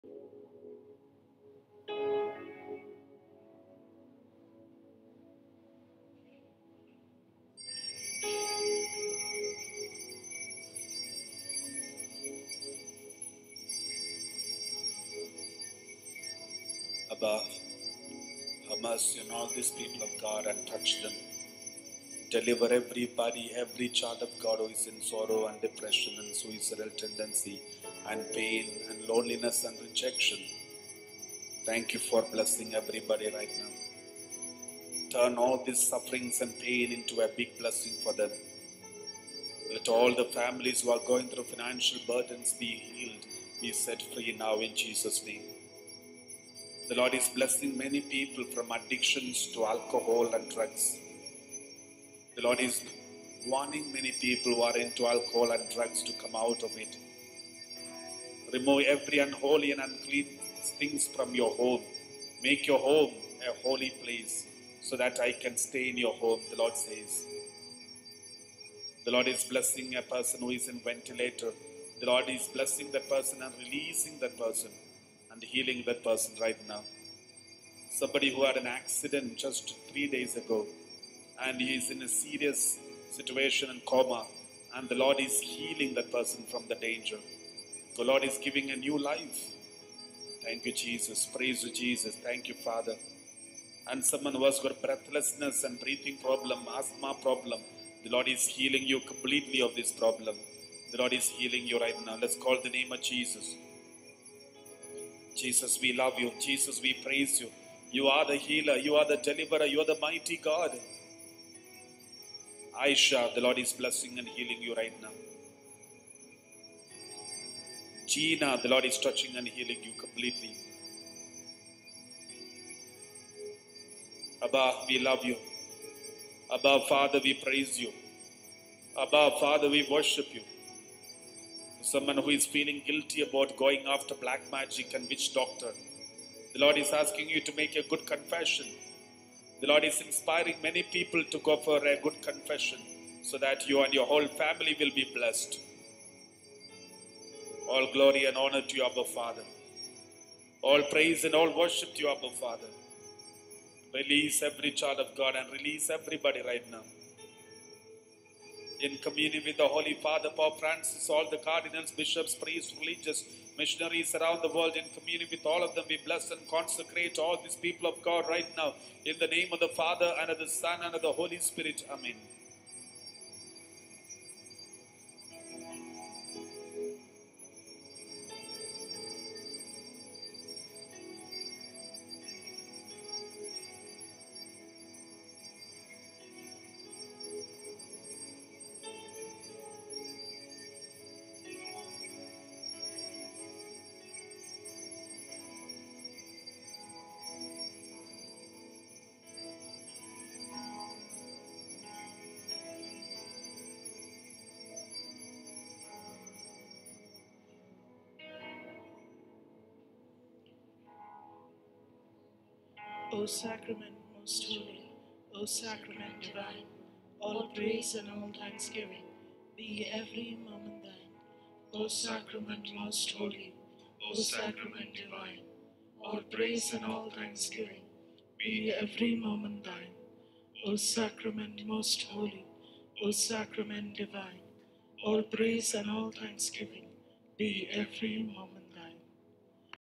May 10 healing and adoration | Divine UK
This video is not owned by me. This is a video of today's Divine UK's healing and adoration. I encourage everyone to join daily and be blessed and healed by the Lord.